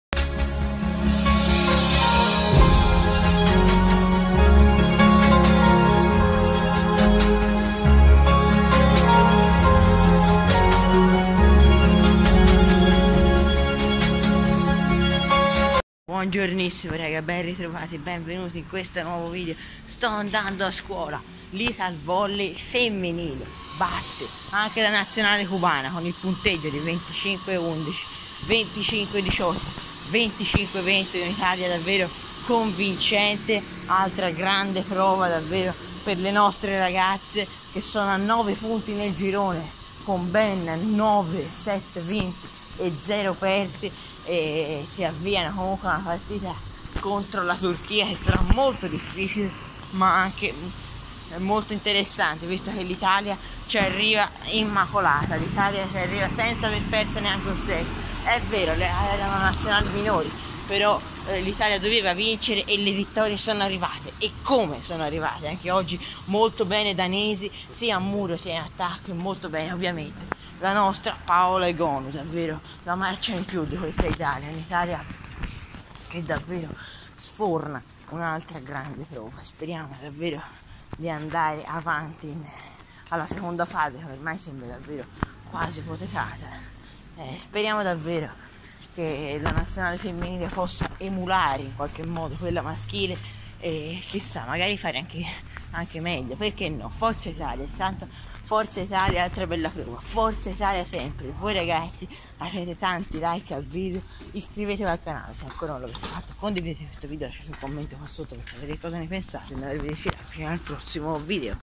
MONDIALI VOLLEY FEMMINILI 2018 : L'ITALIA DEMOLISCE ANCHE CUBA!! TESTA ALLA TURCHIA
Grande partita dell'Italia